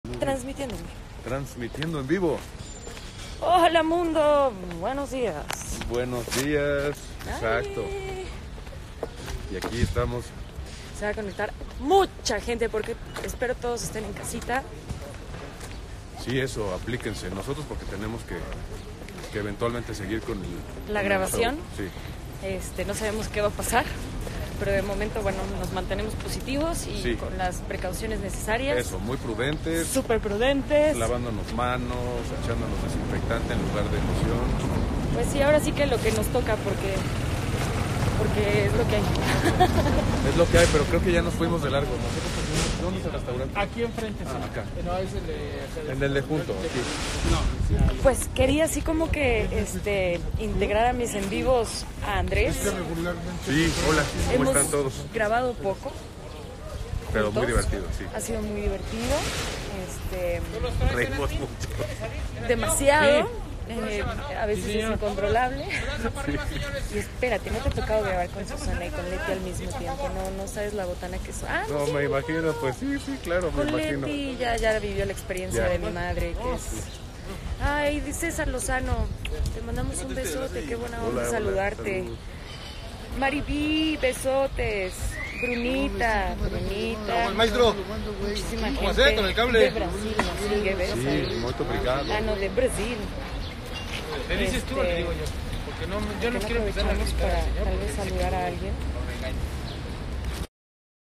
Angelique Boyer - Instagram Live 18 De Marzo De 2020
Grabación integra de la transmisión que hizo en vivo desde su cuenta oficial en instagram la actriz Angelique Boyer el dia 18 de marzo de 2020.